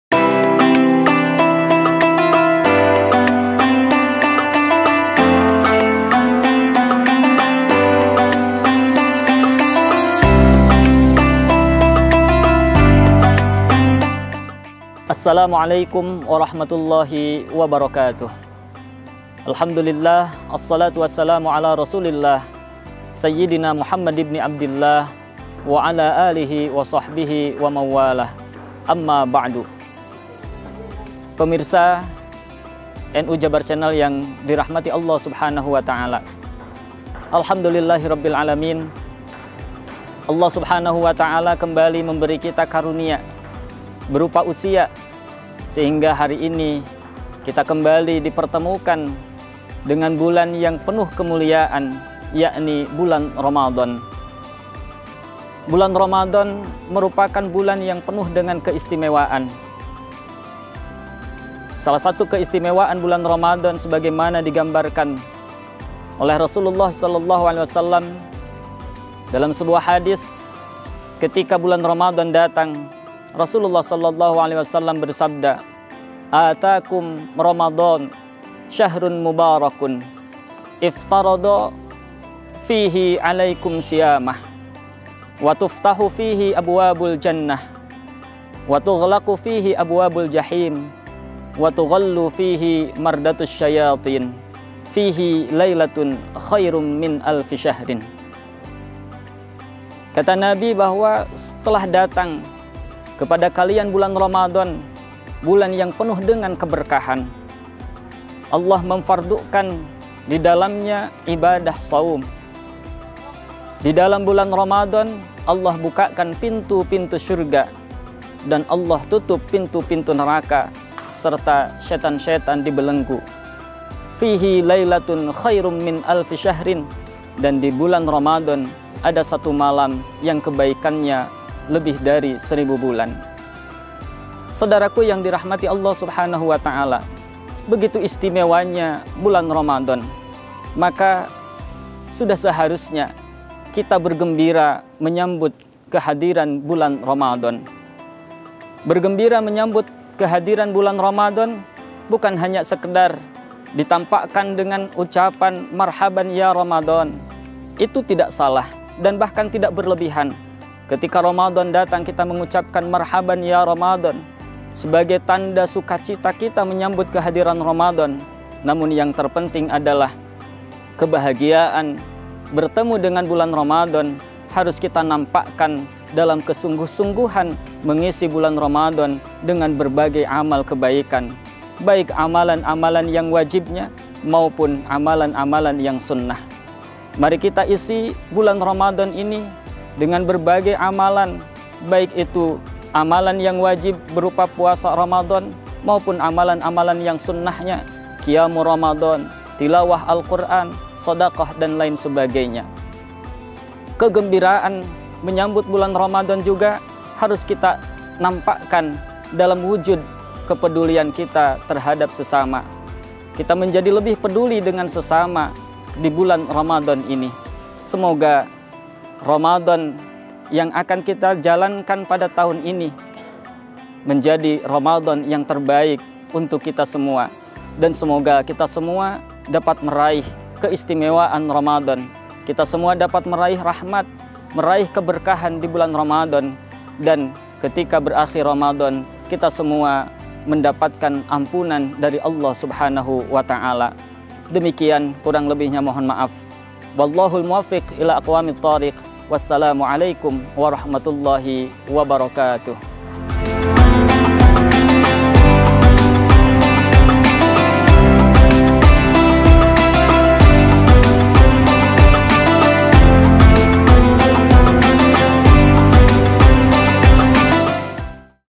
BERGEMBIRA MENYAMBUT RAMADHAN | BERKAH RAMADHAN NGAOS SORE BERSAMA AJENGAN ENDI SUHENDI, M.Pd.
Program BerkahRamadhan​ Tahun 2022 M / 1443 H
Ngaos Sore bersama Kang Ajengan Endi Suhendi, M.Pd.